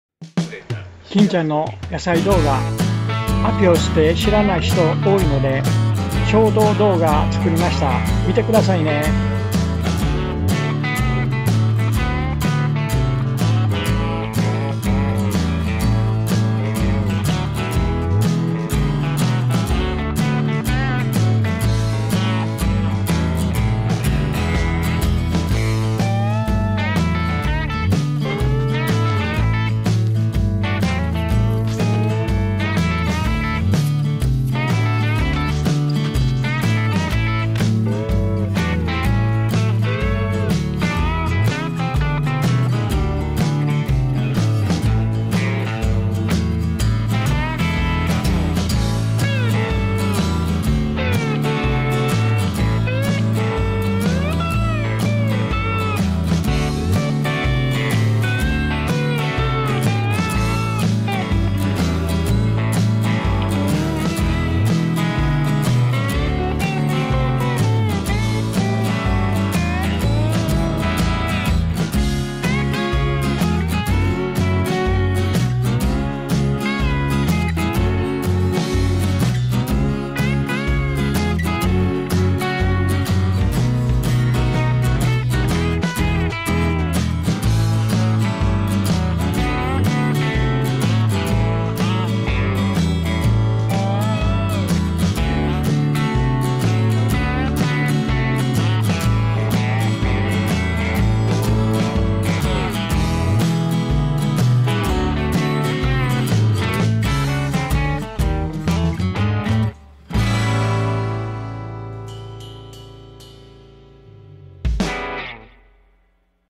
アピオスって何？ 金ちゃん農園ショート①
アピオス　世界三大健康野菜の一つ　菊芋　ヤーコン
アピオスって知らない人が多いのでショート動画で紹介しました。